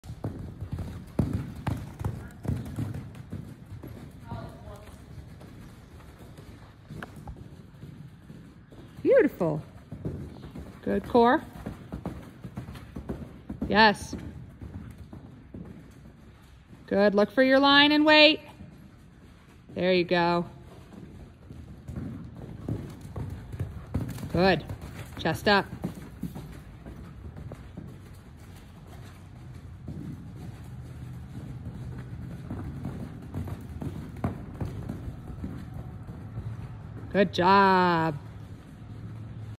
Salsa schooling over fences with teenage rider.
Salsa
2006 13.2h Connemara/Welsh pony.